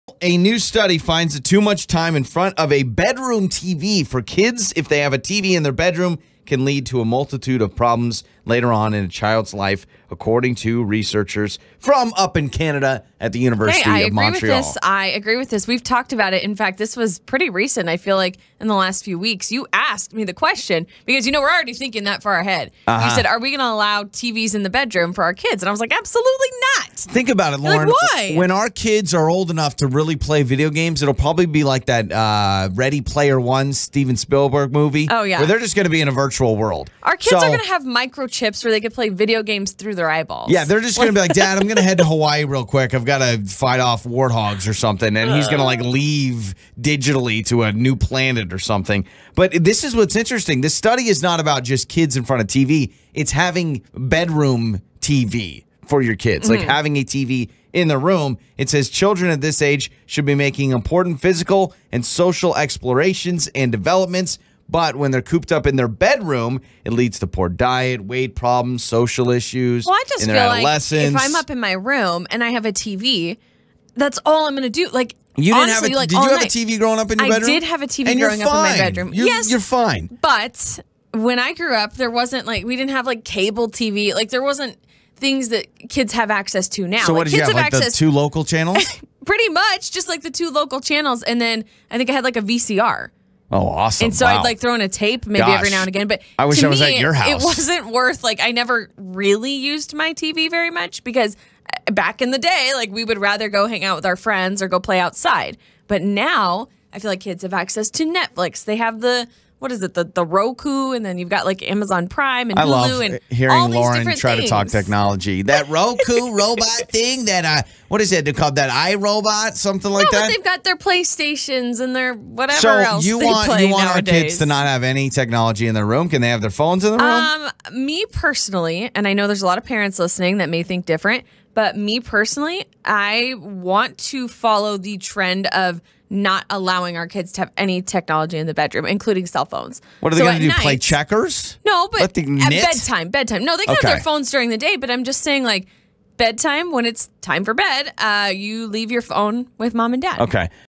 TV in your kids bedrooms
According to researchers from up in Canada at the University of Montreal, a new study finds that kids that spend too much time in front of their bedroom TV can lead to a multitude of problems later on in their life...